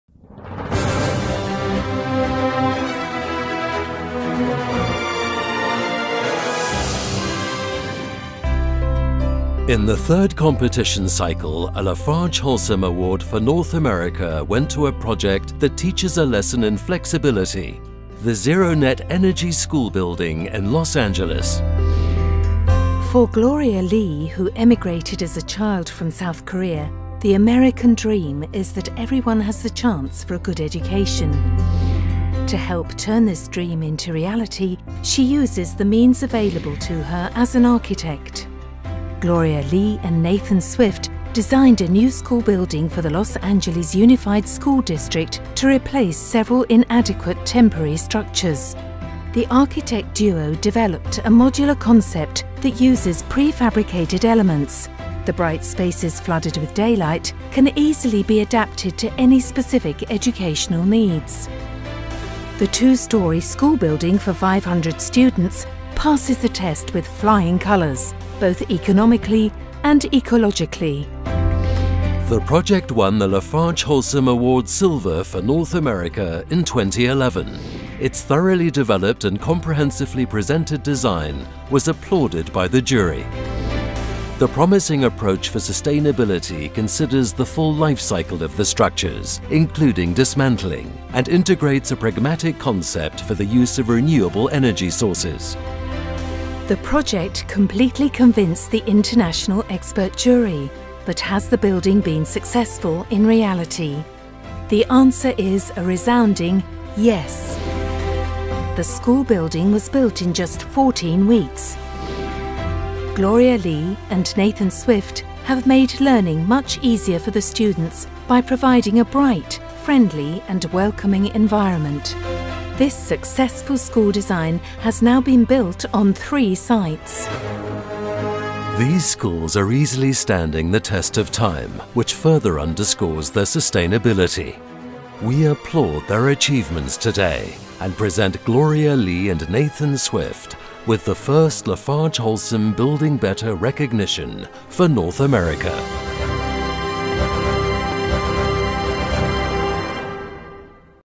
Zero net energy school building: Building Better Recognition 2017 North America
The LafargeHolcim Building Better Recognition in North America went to Gloria Lee and Nathan Swift of SwiftLeeOffice in Pasadena, CA, USA. The two architects designed a new school building prototype for the Los Angeles Unified School District to replace several inadequate temporary structures.